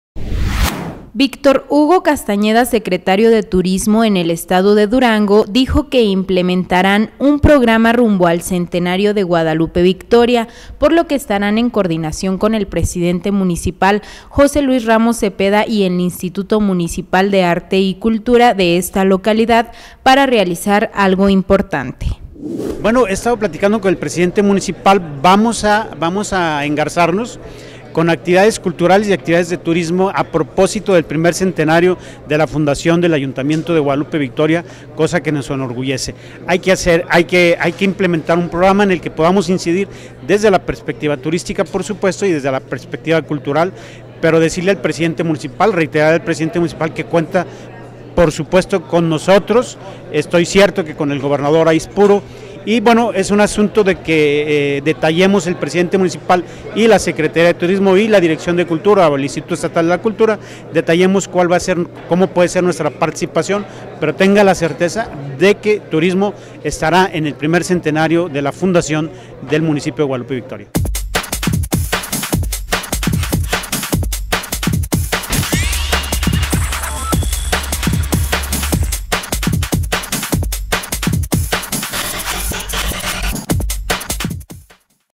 Guadalupe Victoria | Proyecto de turismo rumbo al bicentenario de GV
Guadalupe Victoria, Dgo.- Víctor Hugo Castañeda,  secretario de turismo en el estado, dijo que implementaron un programa rumbo al centenario de Guadalupe Victoria, por lo que estarán en coordinación con el presidente municipal y la dirección de cultura de esta localidad para realizar algo importante ese día.